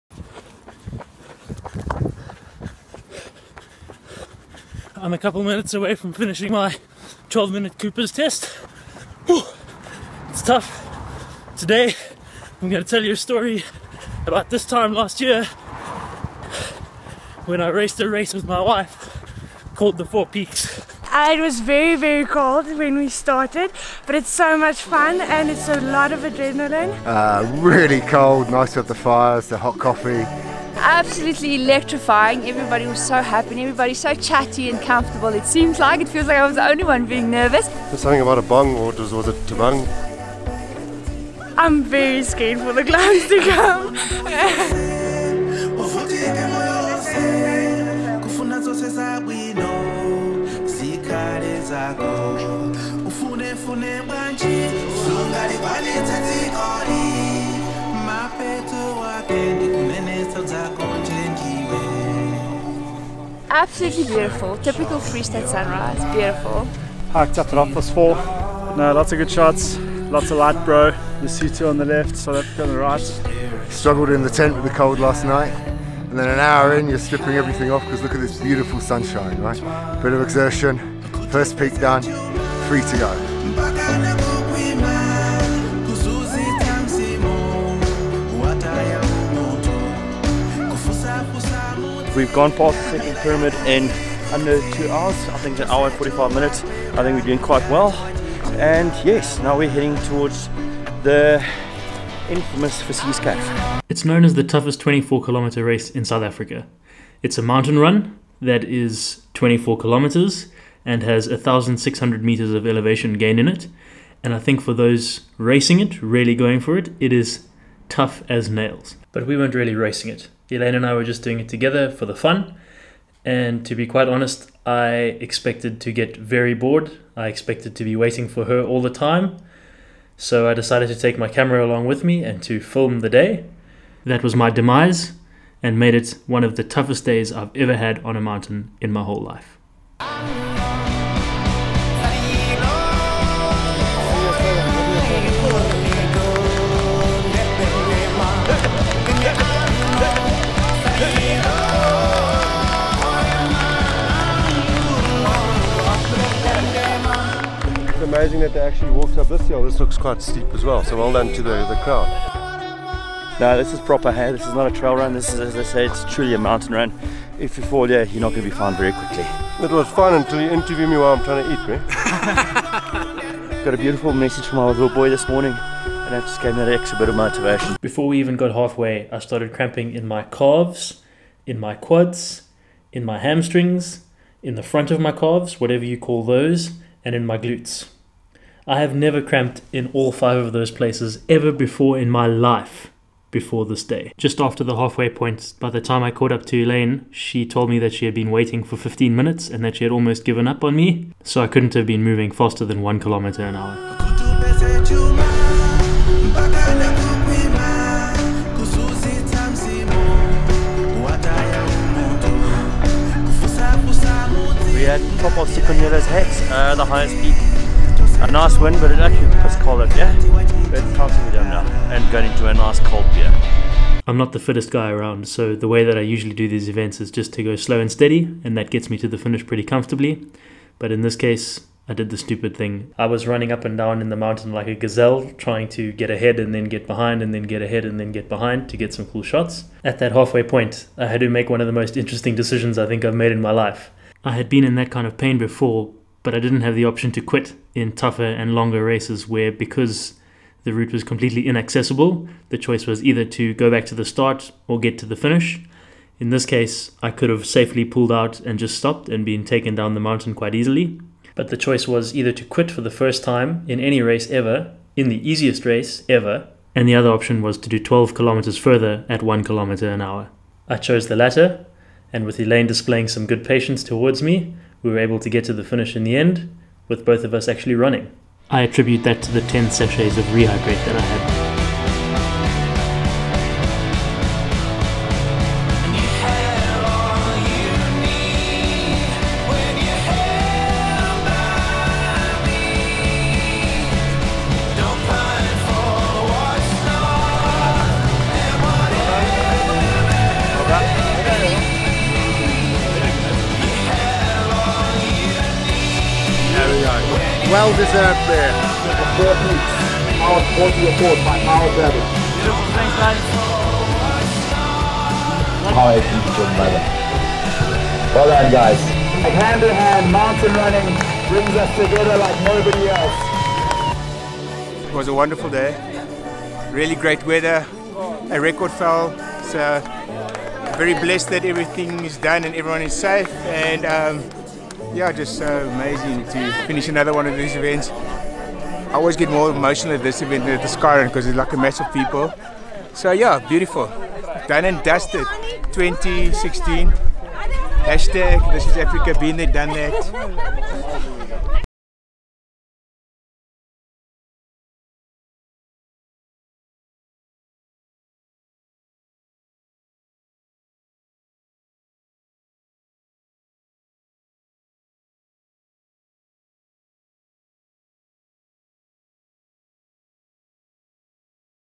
My 4PEAKS Story
My 4PEAKS story is about the time that Elaine and I did a mountain race together.